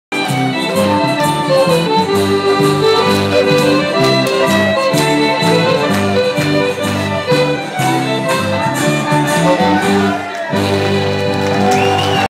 Medieval Banquet, Festival of Falconry '09
The last of the conga line, spot Ben, our token hippy, at the front!